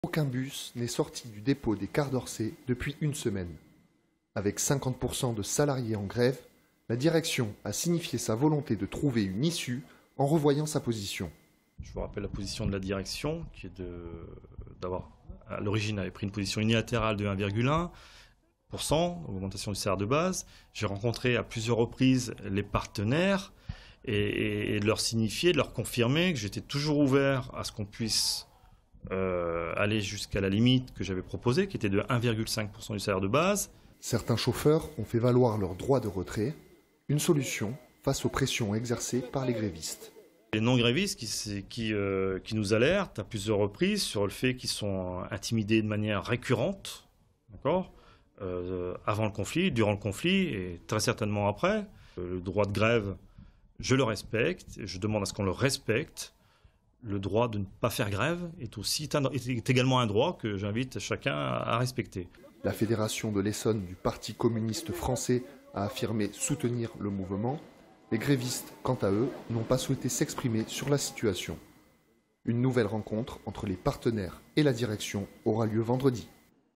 La grève se poursuit chez Les Cars d'Orsay
Résumé de la situation chez les Cars d'Orsay avec la grève qui se poursuit pour une augmentation salariale.
Sites webs/blogs, copiez cette vidéo avec le bouton "Intégrer" !